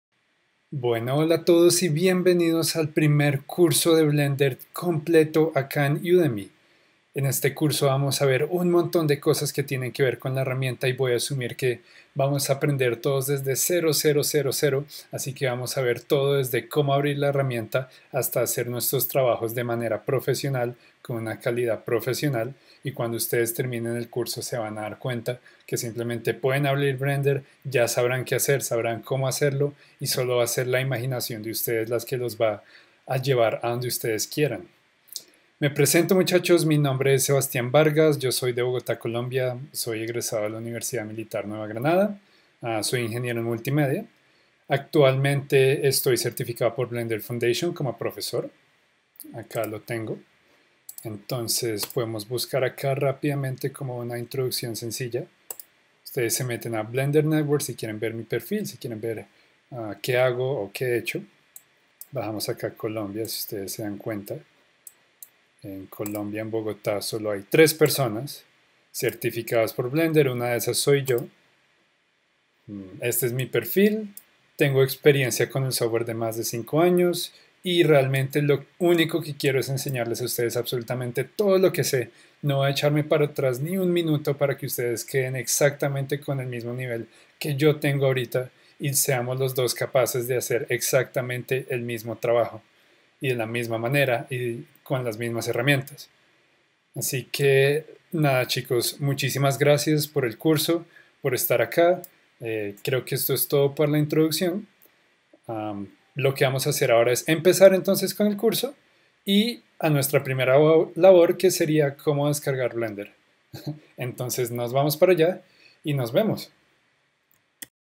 Blender Curso Completo + Integración WEB & Unreal Engine
¿Quieres empezar a trabajar con Blender? ¿Deseas saber acerca de Modelado, Animación, Render y todo lo demás que Blender te ofrece? ¿Quieres utilizar Unreal Engine para hacer visualizaciones profesionales? ¡Este curso es para ti!

¡No debes tener experiencia previa en absoluto! Aprenderás toda la interfaz, Modelado tridimensional, Animación Tridimensional, Texturizado, Iluminación, y Render (Con los dos Motores de render que trae el software y el motor de juegos Unreal Engine).

Haremos varios ejercicios, veremos técnicas de la industria y utilizaremos ADDONS imprescindibles que permitirán que tu trabajo tenga la máxima calidad posible. Mejor aún, ¡Encontraste el curso más completo y más económico de Blender en todo Udemy en español!

¿Qué esperas? ¡Acá estás Bienvenido!
¿A quién está dirigido?

    Cualquier persona que desee aprender Blender desde 0.
    Cualquier persona que desee profundizar sus conocimientos en Blender.
    Alguien que venga de otros software 3D como Maya, 3D Max, Cinema4D, etc.
    Cualquier persona que desee entender los conceptos de computación gráfica requeridos por el software.